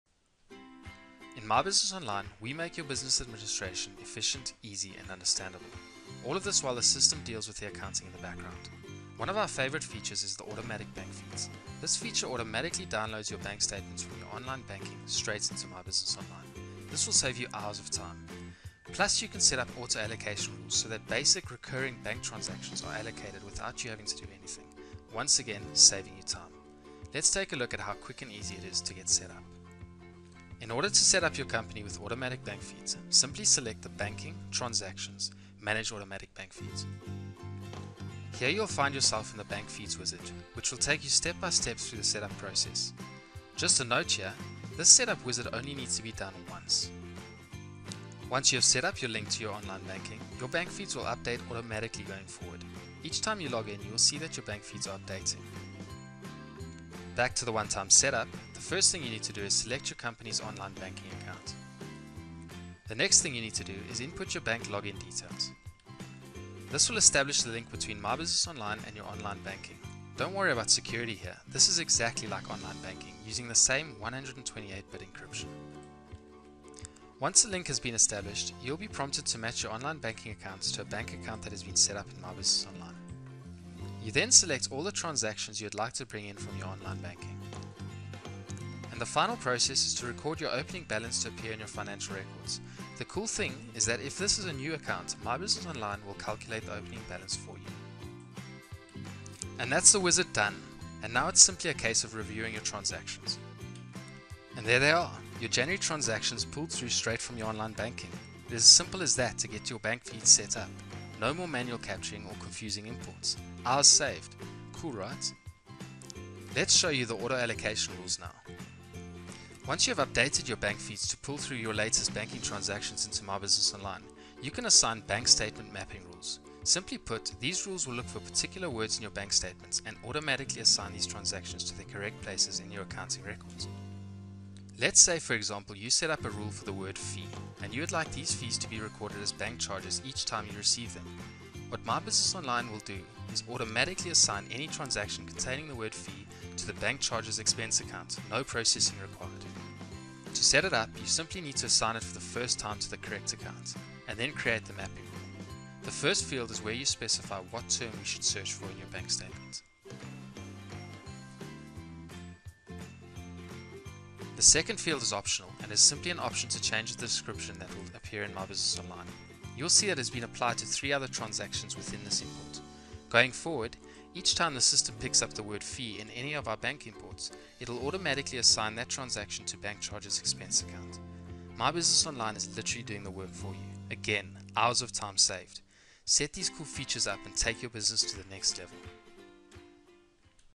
Sage Pastel Online - Automatic Bank Feeds
Easily and Automatically import your bank and credit card feeds into My Business Online.

Simple and secure online accounting for small and start-up businesses.